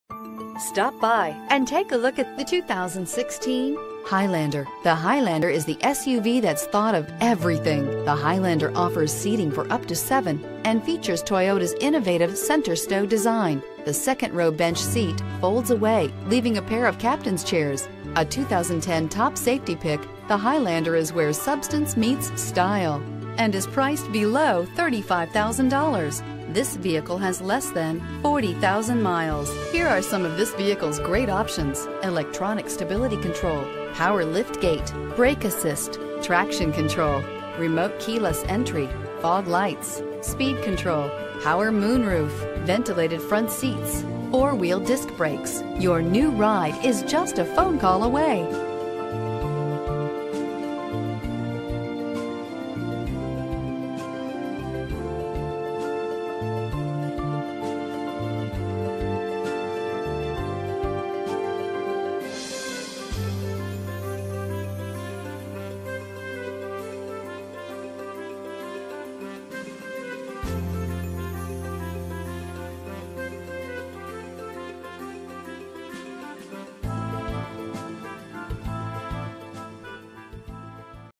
2016 Toyota Highlander Elk Grove Toyota E25740
2016 Toyota Highlander  - Stock#: E25740 - VIN#: 5TDYKRFH2GS135972

Check out this 2016 Toyota Highlander.   Recent Arrival! BEAUTIFUL CONDITION LOADED, QUAD SEATING, MOON ROOF, NAVIGATION, Certified. Predawn Gray Mica 2016 4D Sport Utility Toyota Highlander Limited 19/25 City/Highway MPG FWD 6-Speed Automatic Electronic with Overdrive 3.5L V6 DOHC Dual VVT-i 24V Odometer is 1691 miles below market average!brbrToyota Certified Used Vehicles Details:brbr  * Roadside Assistancebr  * Limited Comprehensive Warranty: 12 Month/12,000 Mile (whichever comes first) from certified purchase date. Roadside Assistance for 1 Yearbr  * 160 Point Inspectionbr  * Vehicle Historybr  * Warranty Deductible: $50br  * Powertrain Limited Warranty: 84 Month/100,000 Mile (whichever comes first) from TCUV purchase datebr  * Transferable WarrantybrbrAwards:br  * 2016 KBB.com 16 Best Family Cars   * 2016 KBB.com Best Buy Awards Finalist Contact the dealership today.